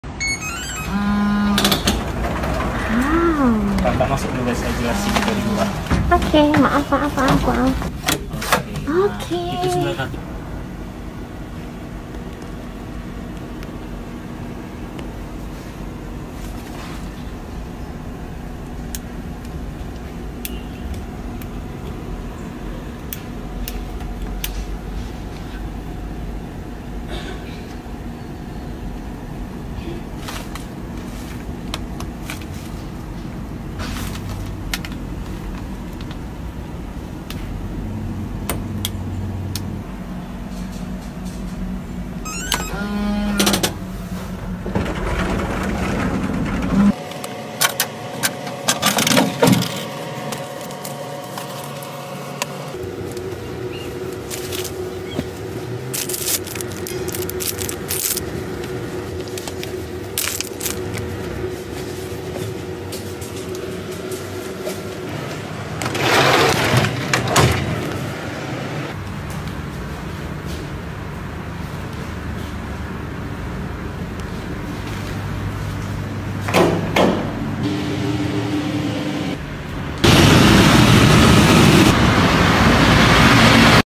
Nginap Nyaman dan Ekonomis di Kota Medan ‘RedDoorz Capsule Inn Medan’
📖 Jejak Langkah O 2023 (Eps. 22)

🔖 Blog: Jejak Langkah O 👉 soon
🔗 soon

Taking video: iPhone 7 Plus

Editing: iPhone 7 Plus, CapCut

🏷 Live in Medan, Prepare to Japan 2023